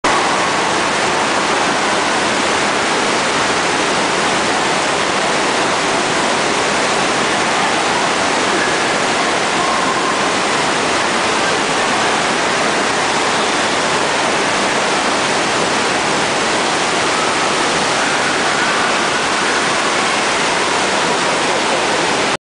cyberjoels indoor surfing
yes you can see the real me surfing in a indoor water park and crashing